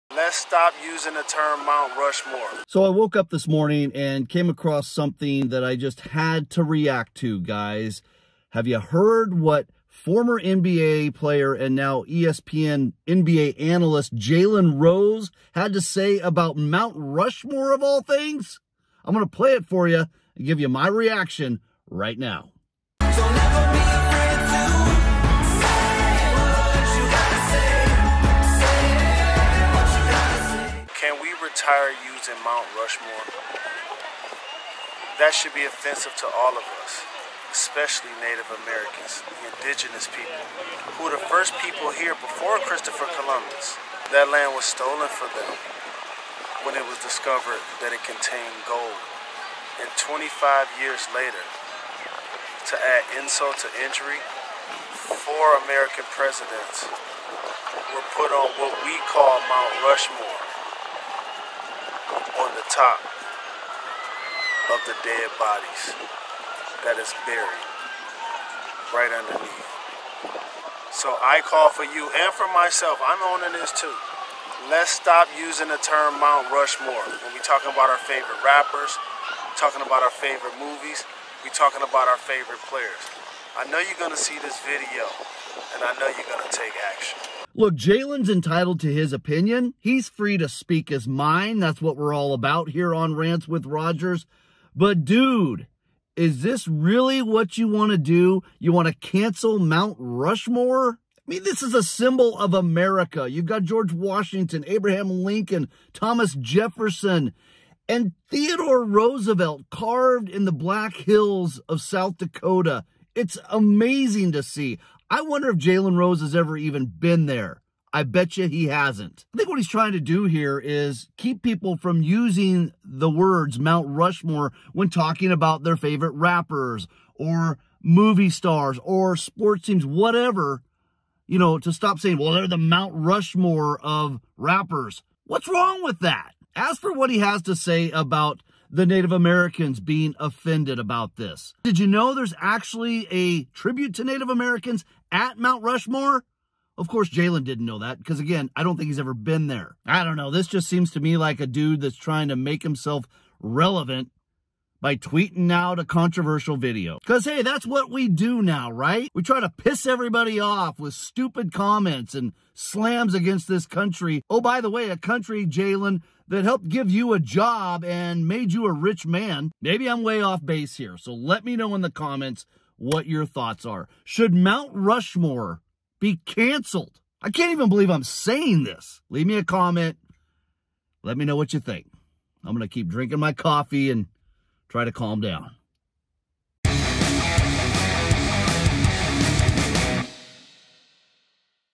Mt. Rushmore CANCELLED? Jalen Rose says YES rant
Why does former NBA player now analyst Jalen Rose want to cancel Mt. Rushmore? He explains it in a video, here it is along with my reaction rant.